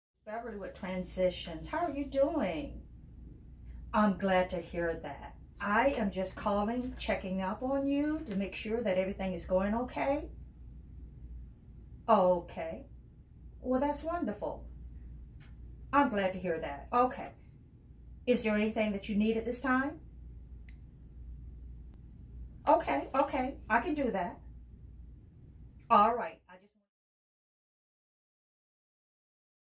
Transitions-Ocala/Marion
Transitions Manager Beverly Crumpler is staying in touch with her current list of 189 clients by phone from her house.